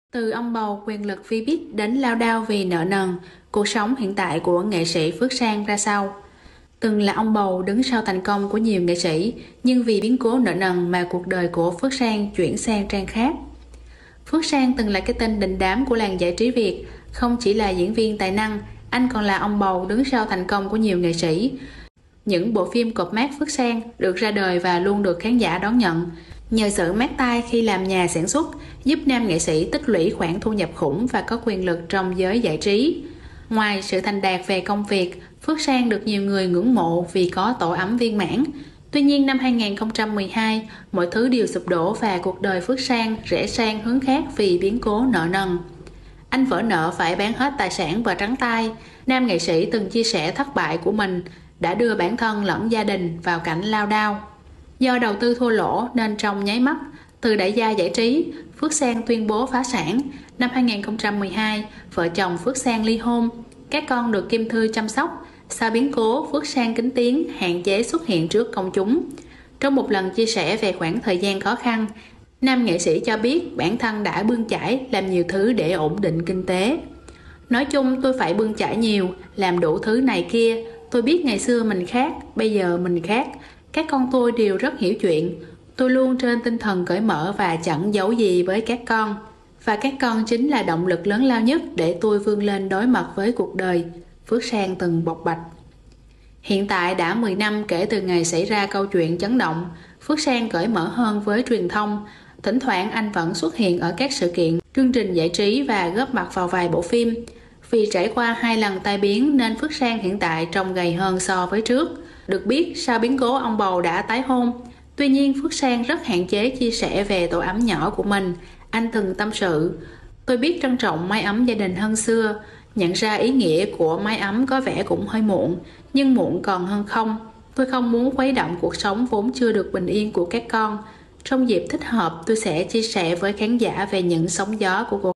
Cuộc đời Phước Sang : Từ ông bầu Tôi thành kẻ trắng tay lúc nào không hay 😢
Phước sang vỡ nợ , Cuộc đời phước sang , ông bầu phước sang

Hiện tại, đã 10 năm kể từ ngày xảy ra câu chuyện chấn động, Phước Sang cởi mở hơn với truyền thông. Thỉnh thoảng, anh vẫn xuất hiện ở các sự kiện, chương trình giải trí và góp mặt vào vài bộ phim. Vì trải qua 2 lần tai biến nên Phước Sang hiện tại trông gầy hơn so với trước.

Được biết, sau biến cố, "ông bầu" đã tái hôn. Tuy nhiên, Phước Sang rất hạn chế chia sẻ về tổ ấm nhỏ của mình. Anh từng tâm sự: "Tôi biết trân trọng mái ấm gia đình hơn xưa. Nhận ra ý nghĩa của mái ấm có vẻ cũng hơi muộn nhưng muộn còn hơn không. Tôi không muốn khuấy động cuộc sống vốn chưa được bình yên của các con. Trong dịp thích hợp, tôi sẽ chia sẻ với khán giả về những sóng gió của đời mình".